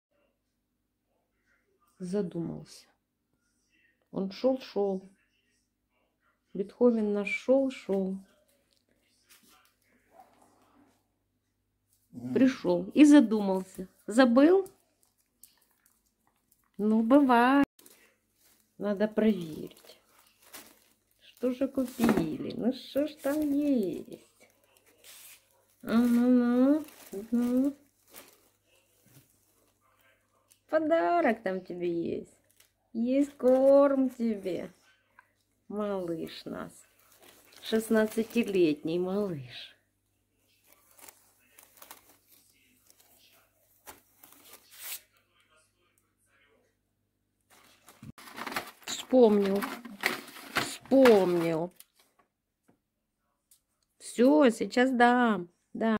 Бетховен задумался
Бетховену 16 лет  он иногда задумывается. идет , идет, а затем стоит, как вкопанный и думает...но с игрушкой еще любит играть и пакеты проверять.